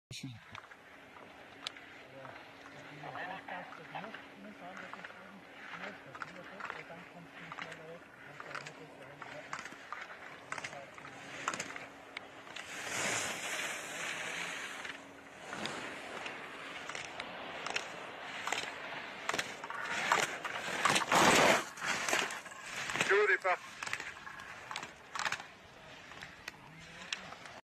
Steven Amiez of France skiing slalom training Saas Fee Switzerland Equipe de Ski Alpin Wintersports
Steven Amiez of France skiing slalom training Saas Fee Switzerland Equipe de Ski Alpin Wintersports - (video by Paco Rassat given to us by Steven Amiez himself ..merci!)
Jeune skieur de l'Equipe de France de Ski Alpin, Steven Amiez is showning some great slalom skills here in training in SaasFee Switzerland, September 2020 glacier alpine skiing training